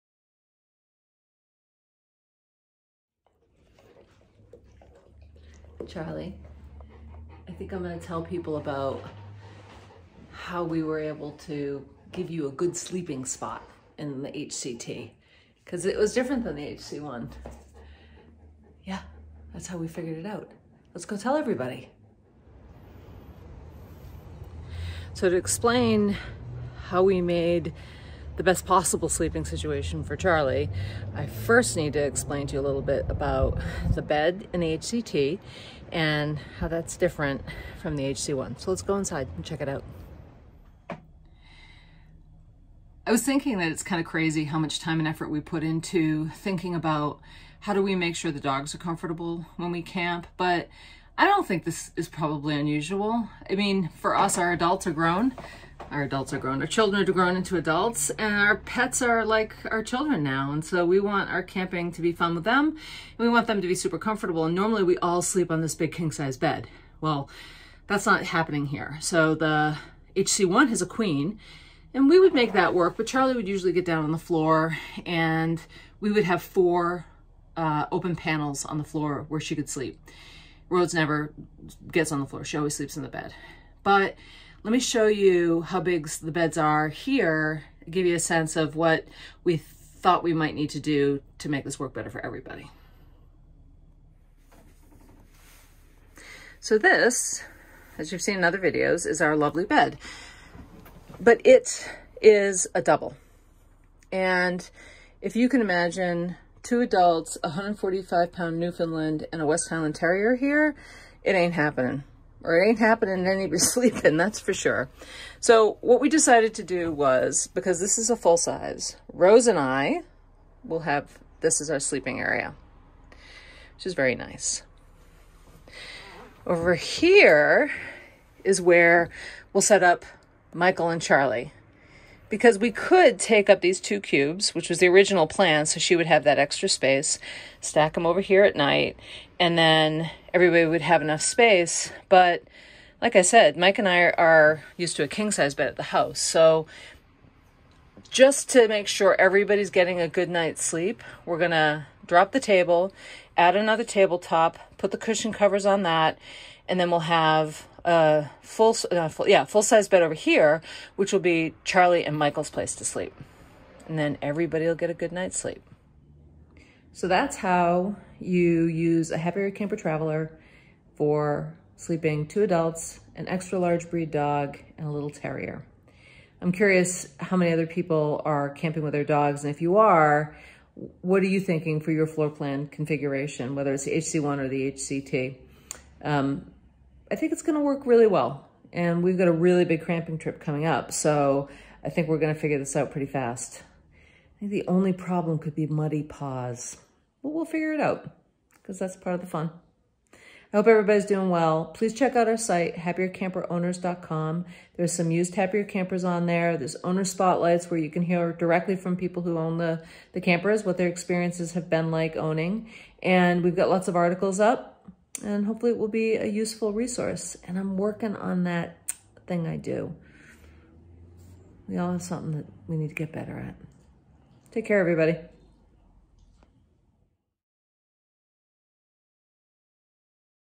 The Happier Camper HCT sleeping layout for two humans, a 145 pd Newf and a West Highland Terrier
Traveling with dogs requires careful planning. Traveling with an extra large breed dog adds a whole other layer of complexity! Here is how we are planning to make sure everyone gets a good night's sleep.